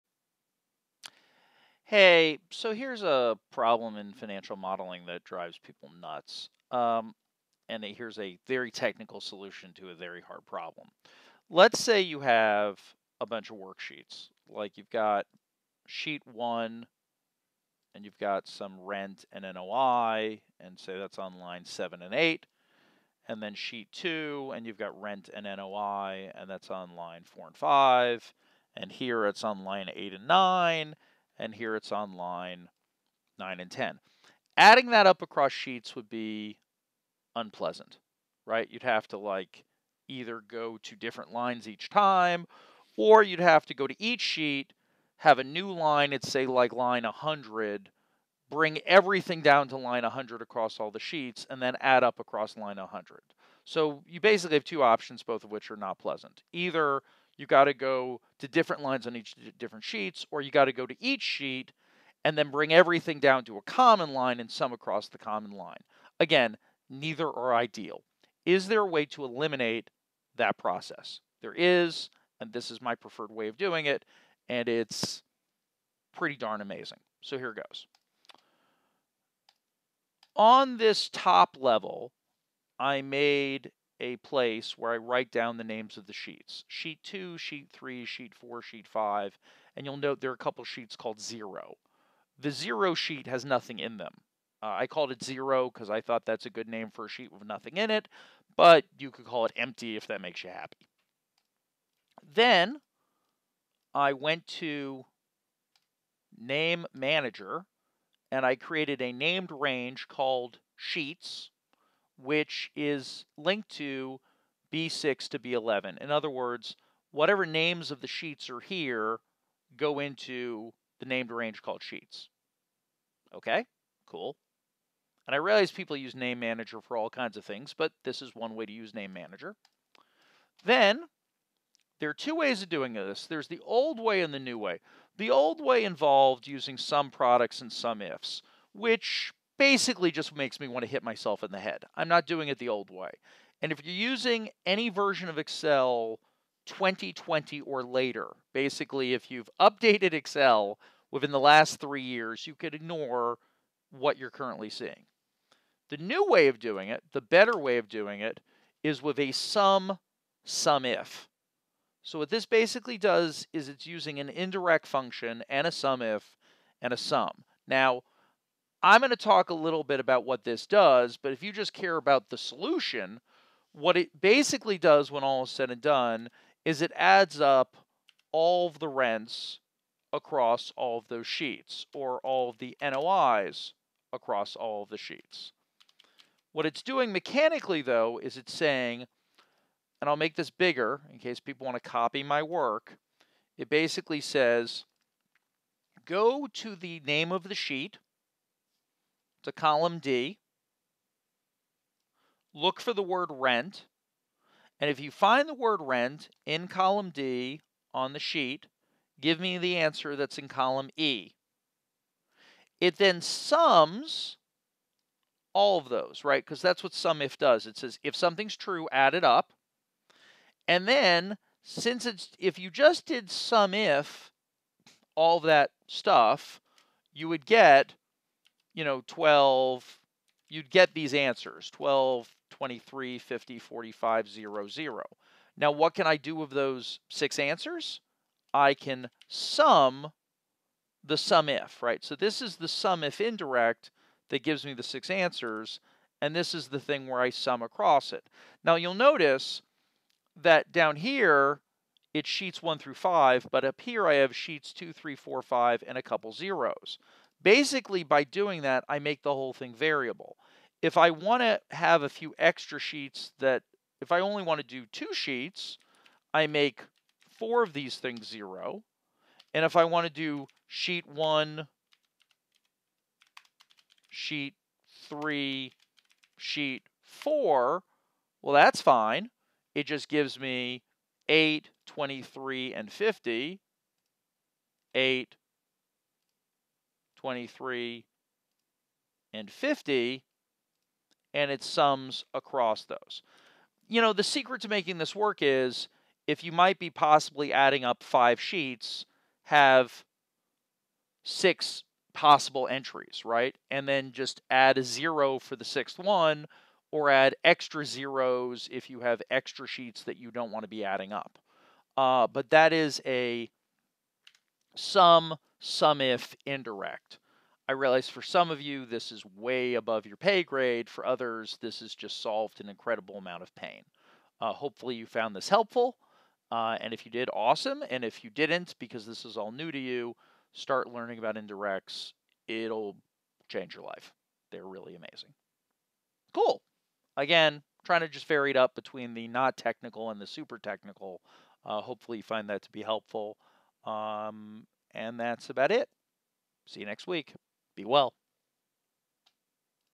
Using SUMIF Across Multiple Worksheets
Need to add up different lines across different worksheets but the lines are in different rows? While there are a lot of ways to solve this problem, here is my preferred solution. Have fun.